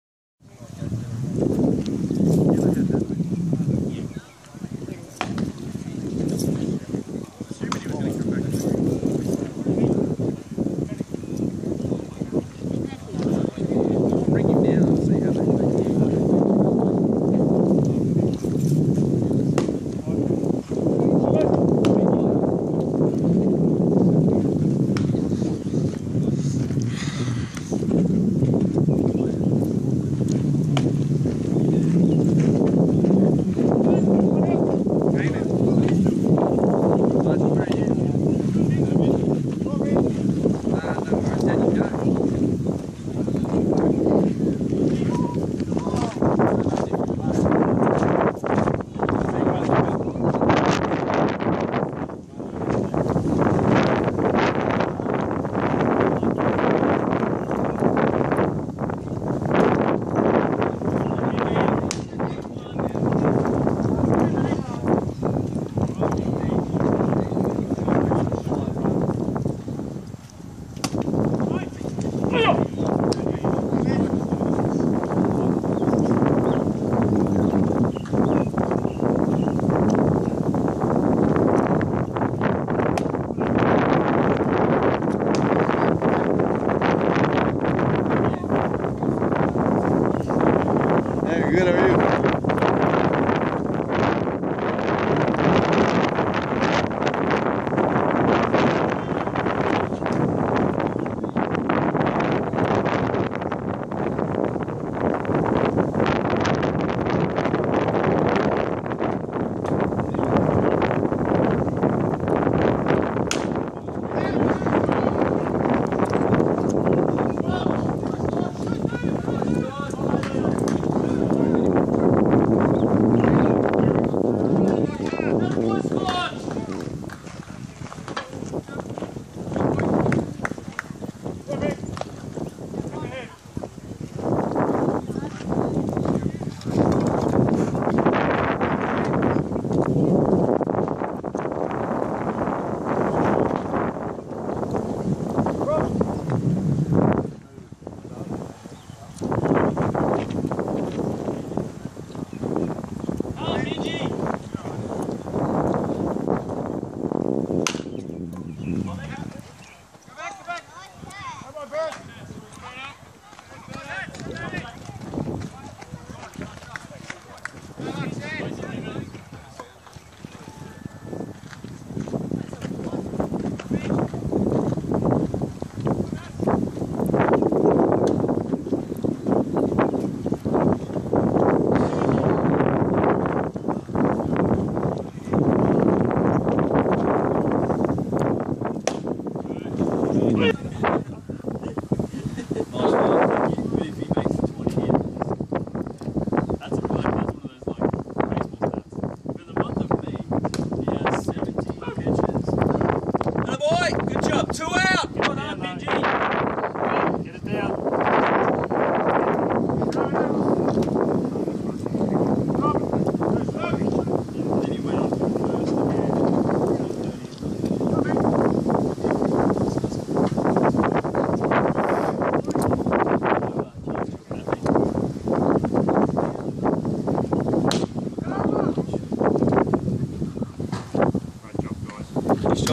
Baseball of Kyeong-Min Kim (20160117)
Kyeong-Min Kim, Melbourne Demons, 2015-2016 Victorian Summer Baseball League In Australia